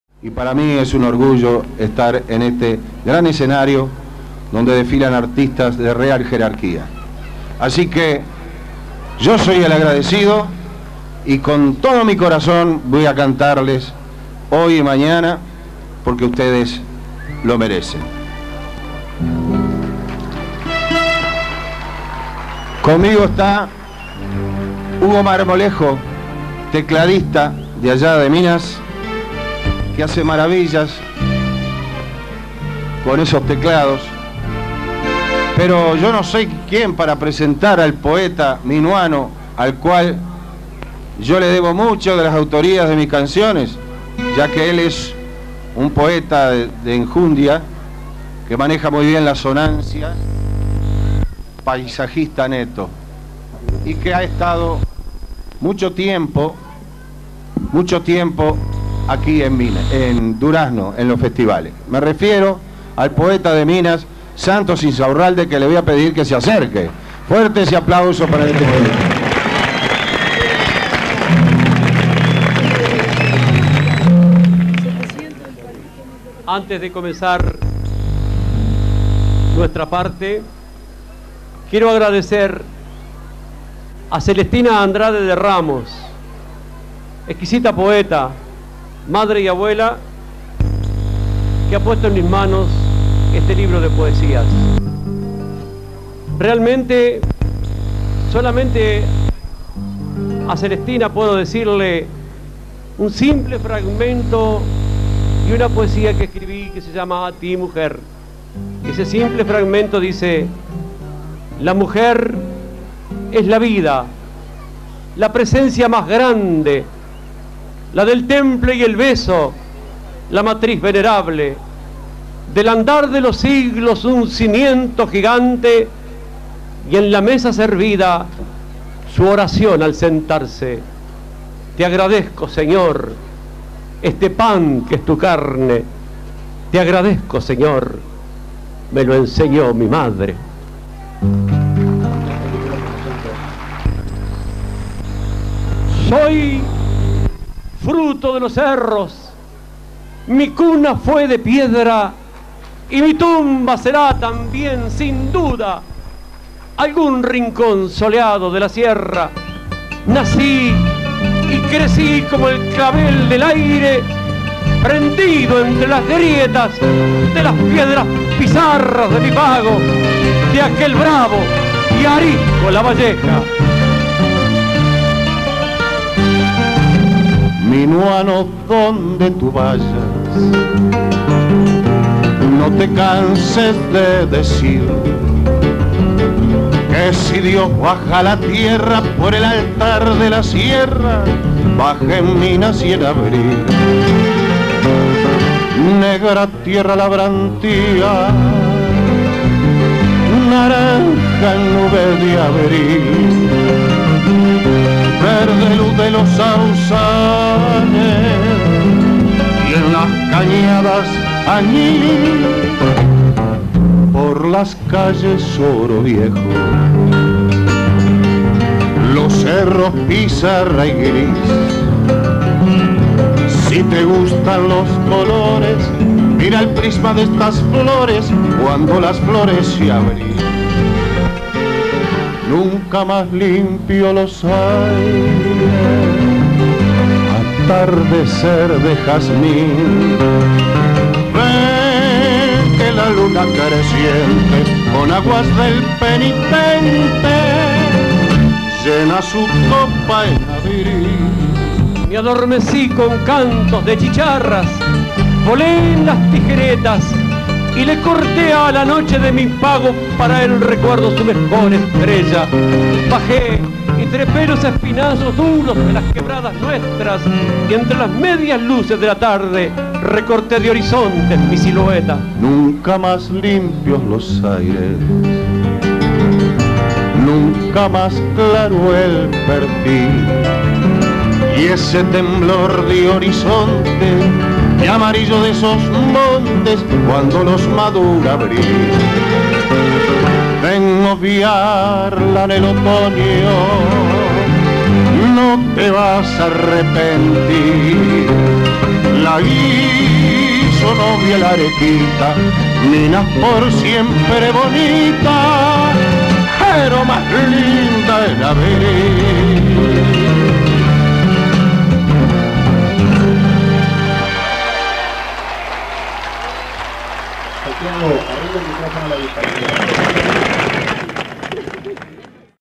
Santiago Chalar - Santos Inzaurralde - Parte 1
En este video se ve a dos grandes exponentes de la cultura del Uruguay. El cantante y compositor Santiago Charlar y el poeta Santos Inzaurralde, actuando para el festival "Todo el Uruguay Canta en Durazno" llevado a cabo en los primeros años de la década del noventa.
Ambos artistas nos narraron mayoritariamente en sus obras, los encantos y pasisajes de los suelos del departamento de Lavalleja y de las sierras minuanas. Santos Inzaurralde nació en Minas; en cambio Santigo Chalar, médico de profesión, nació en Montevideo, capital del Uruguay, pero el tiempo lo llevó a radicarse en Minas y allí desarrollar su carrera profesional y artística.